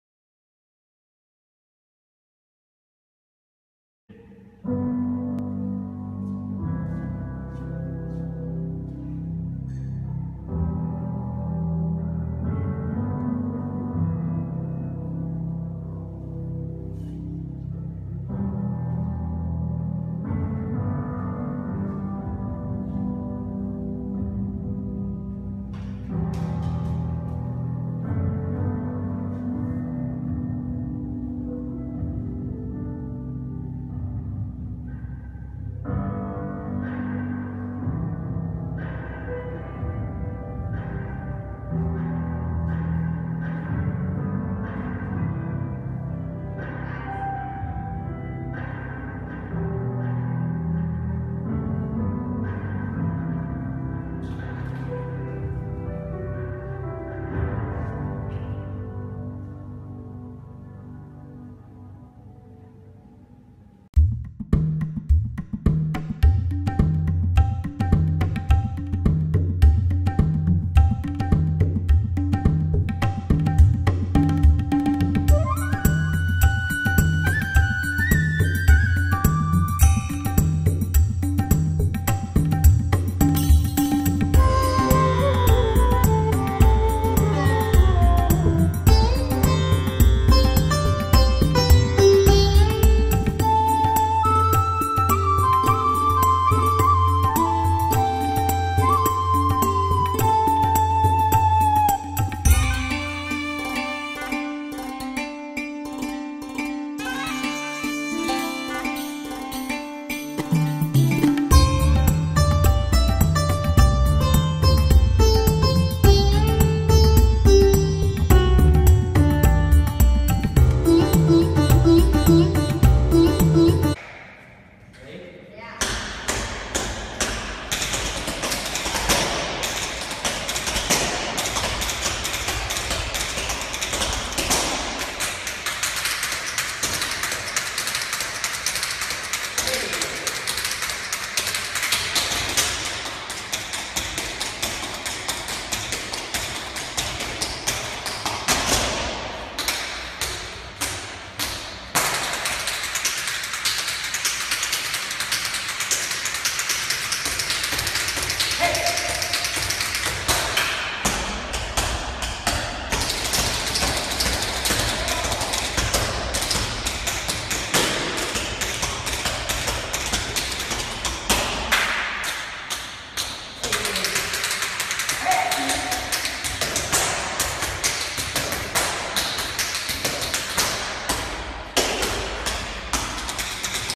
BTS Dance Videos - Showstopper Sparkle Society Photoshoot
Here's a look at some of the dances that are being done at the Sparkle Society photoshoot! This isn't everything, I'm excited for more!

Also please note Brooke's video didn't have sound on it so I added a random sound file from iMovie and it just kinda worked! lol